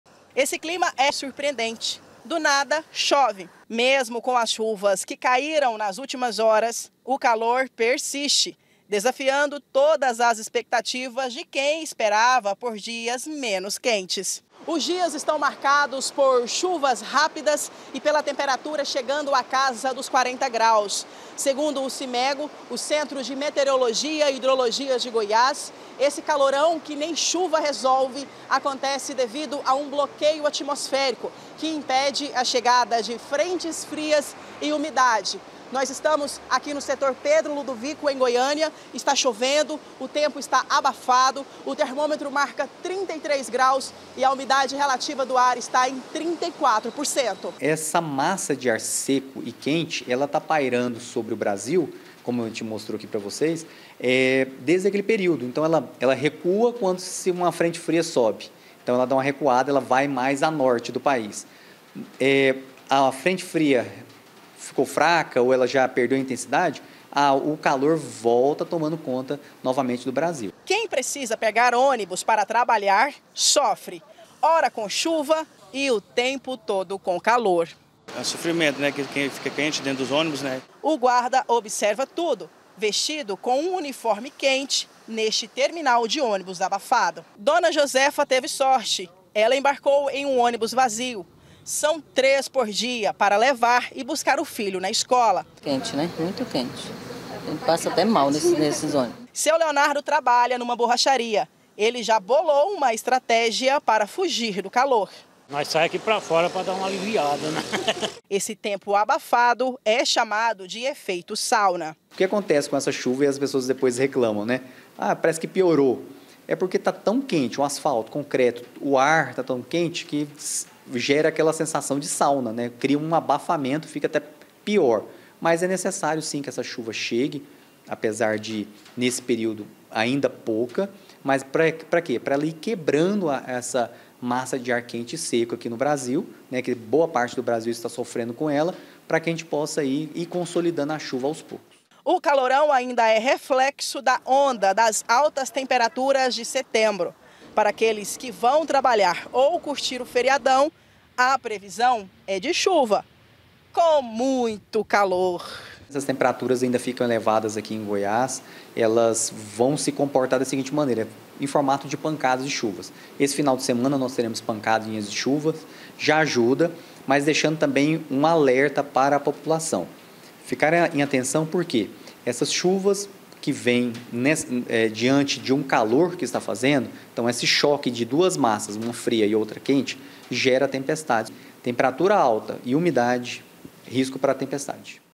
CALOR PERSISTENTE: ALTAS TEMPERATURAS CONTINUAM MESMO COM AS CHUVAS
MESMO COM CHUVA, O CALORÃO SE APROXIMA DOS 40 GRAUS. É QUE CHOVE TÃO POUCO QUE NÃO É SUFICIENTE PRA REFRESCAR. SOBE AQUELE MORMAÇO E DÁ UM EFEITO SAUNA. DE ACORDO COM A METEOROLOGIA, CHUVA MAIS INTENSA SÓ NO FIM DA SEMANA QUE VEM.